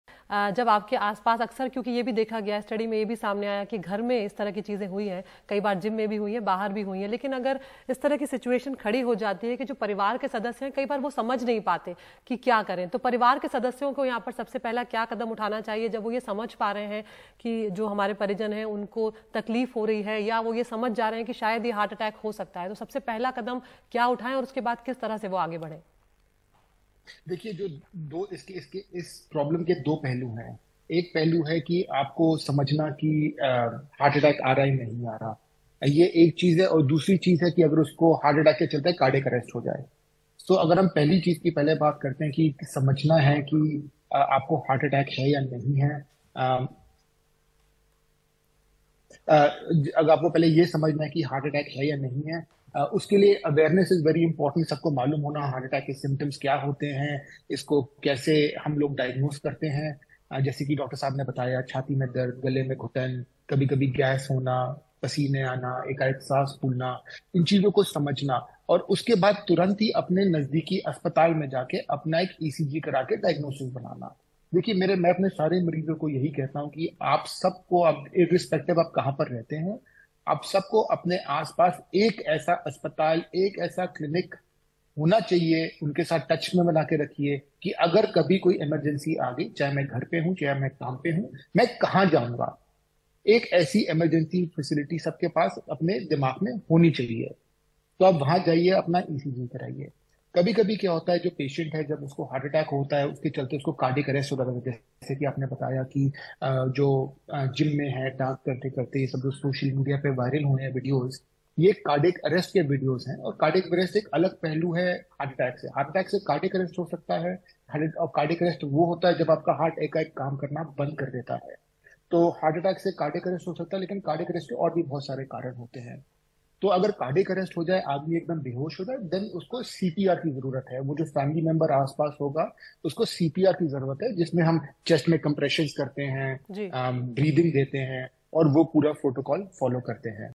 टल जाएगा Heart Attack? Expert से जानिए पूरा मामला | Sawaal India Ka
हार्ट अटैक (Heart Attack) से होने वाली आधे से अधिक मौतें इसलिए होती हैं, क्योंकि लोगों को उन संकेतों और लक्षणों के बारे में पता नहीं होता है, जिन पर गोल्डन आवर (Golden Hour) के दौरान ध्यान देने की आवश्यकता होती है. एक्सपर्ट ने बताया कि हार्ट अटैक से पहले शरीर में कुछ संकेत देता है, जिसे नजरअंदाज नहीं करना चाहिए..एक्सपर्ट से जानिए पूरा मामला.